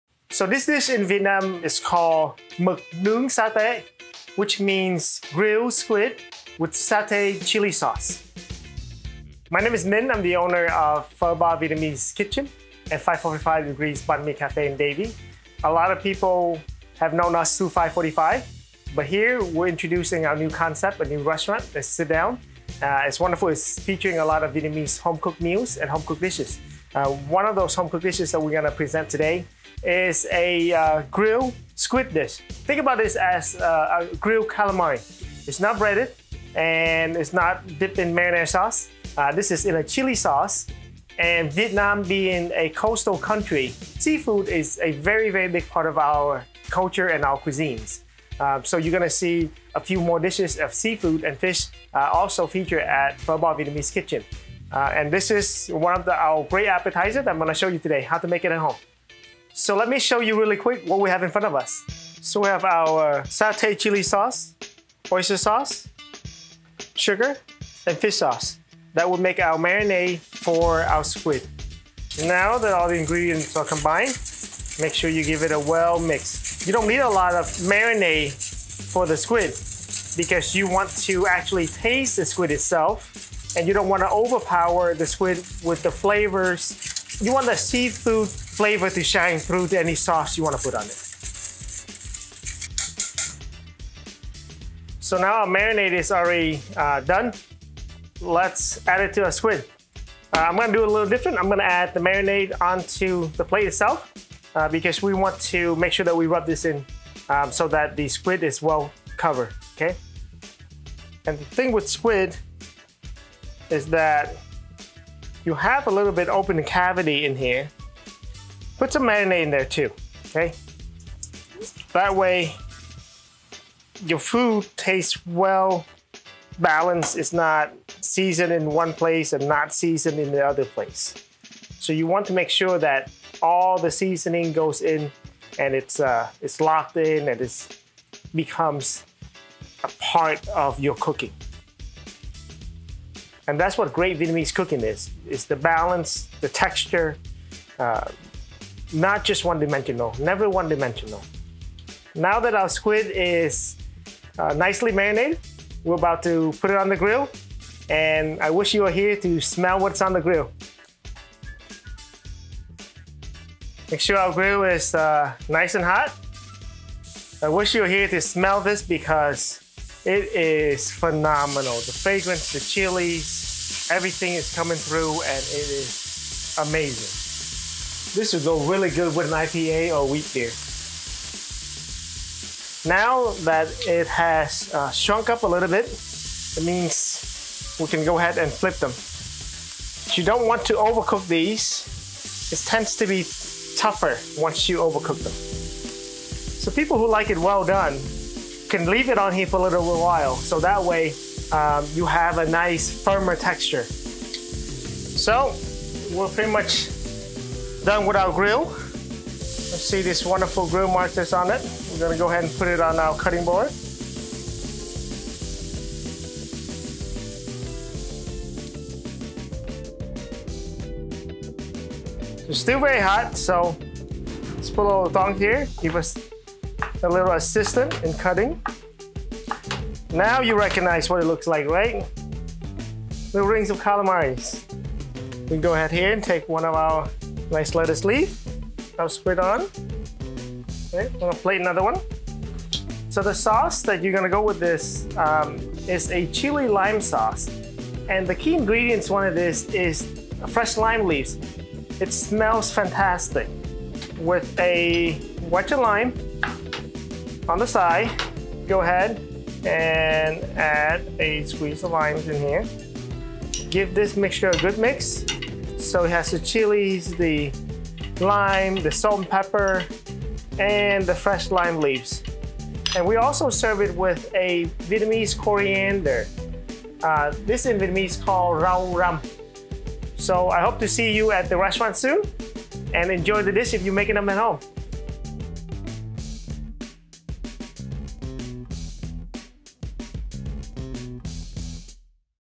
Pho Bar Vietnamese Kitchen - Grilled Squid
Restauranteur Chef Minh Do prepares his favorite Vietnamese recipes to be featured at south Florida’s newest dining experience, Pho Bar.

Recipe #3 is a Grilled Squid dish called Mực Nướng Sa Tế!  The Vietnamese version of calamari that will leave your tastebuds tingling and wanting more.